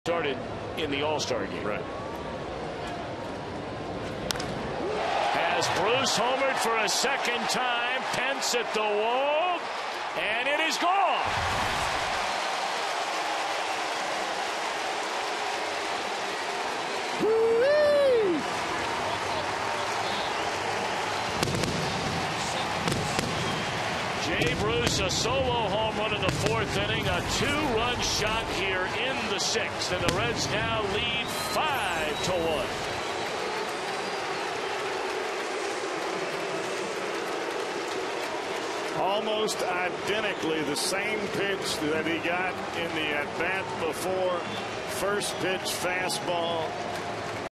2009/05/27 Bruce's second blast
HOU@CIN: Bruce's second homer extends lead to four